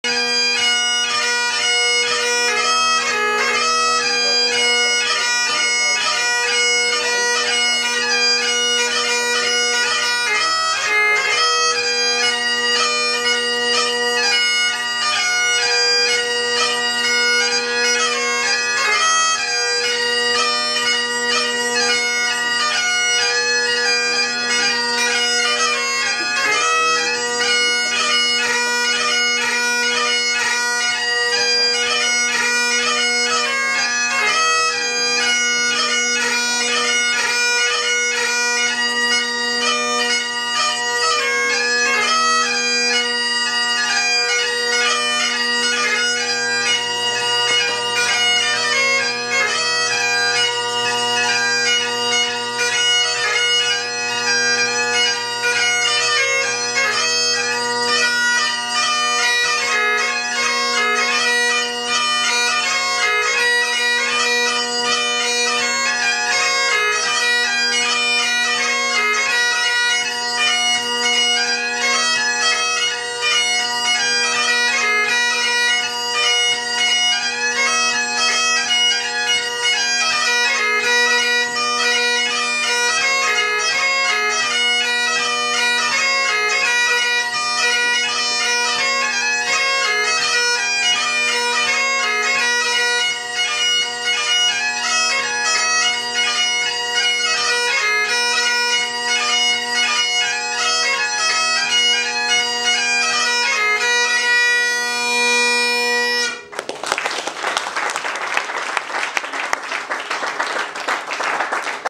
Michael Gray 5th October 2010
Michael with some Strathspreys and Reels